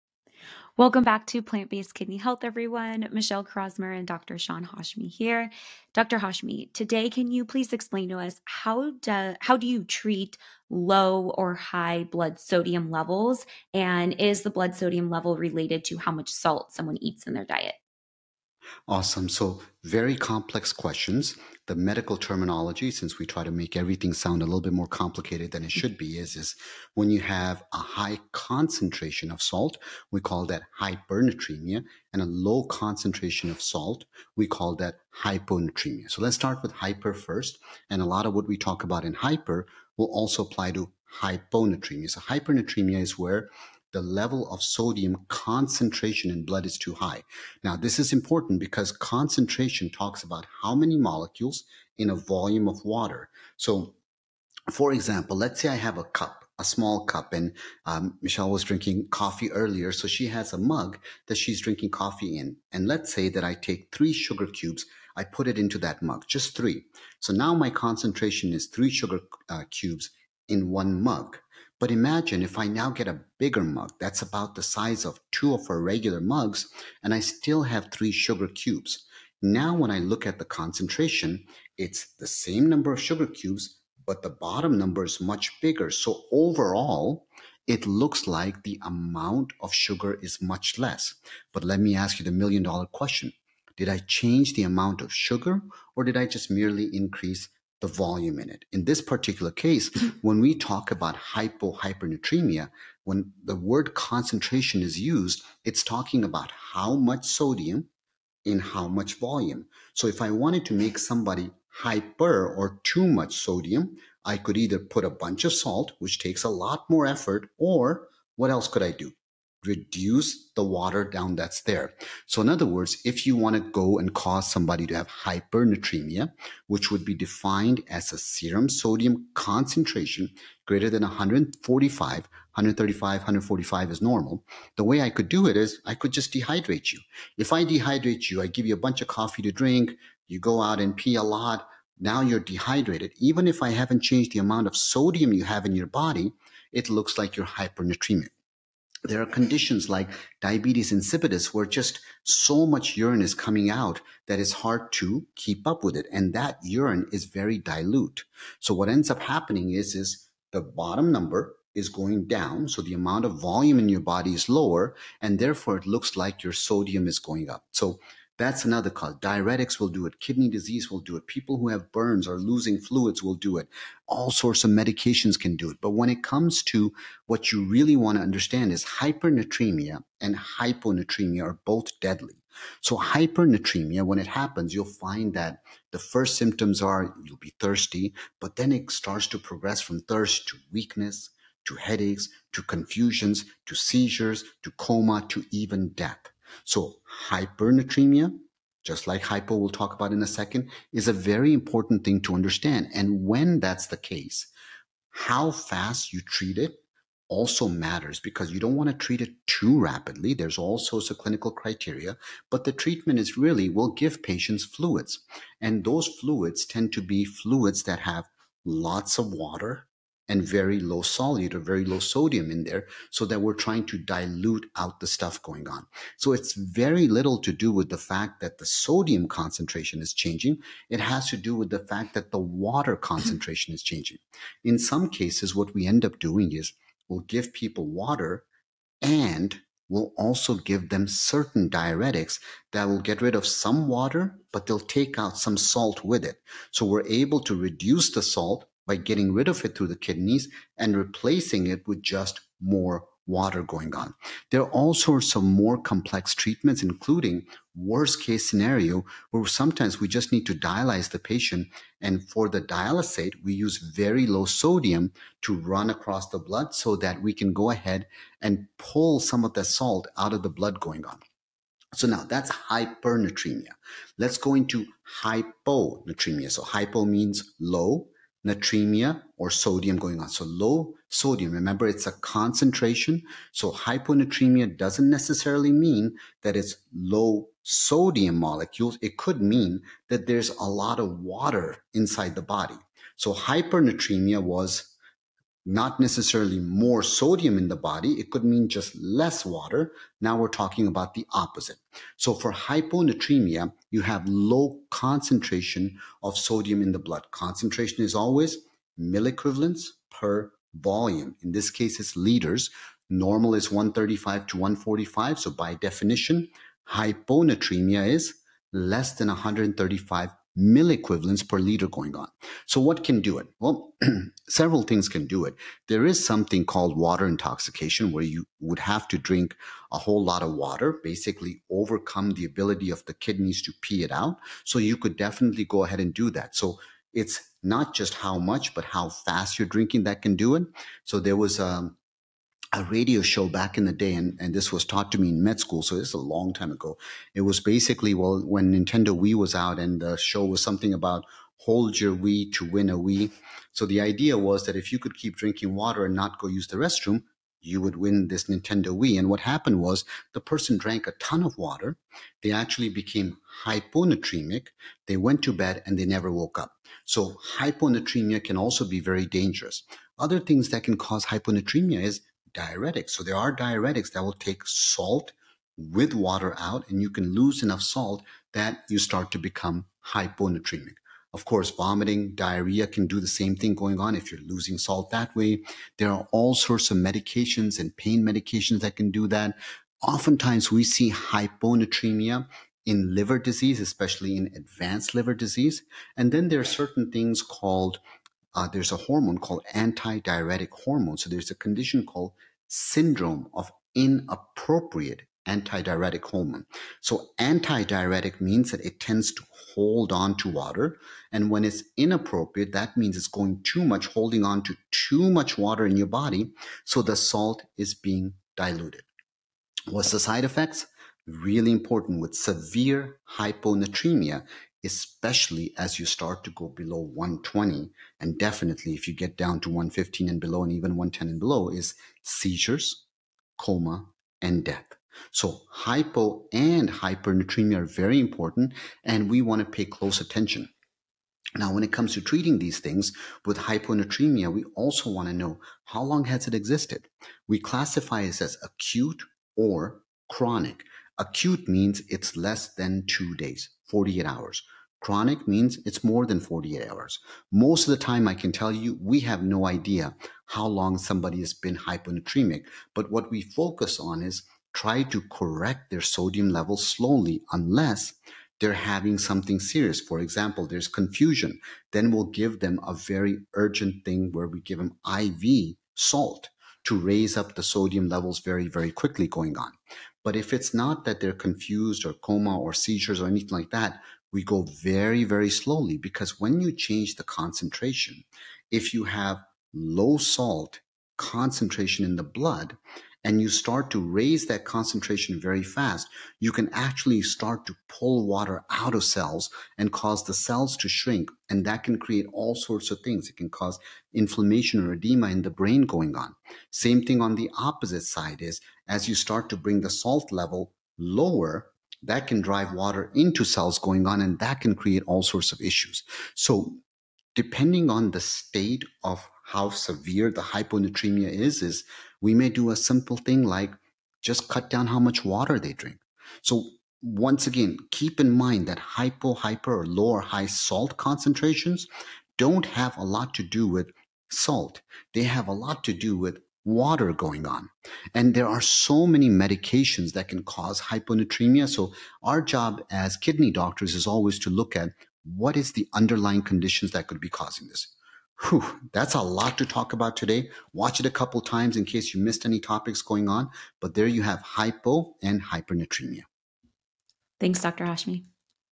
Low & High Blood Sodium Levels: Hyponatremia & Hypernatremia-How To Treat
Sean Hashmi, MD, MS, FASN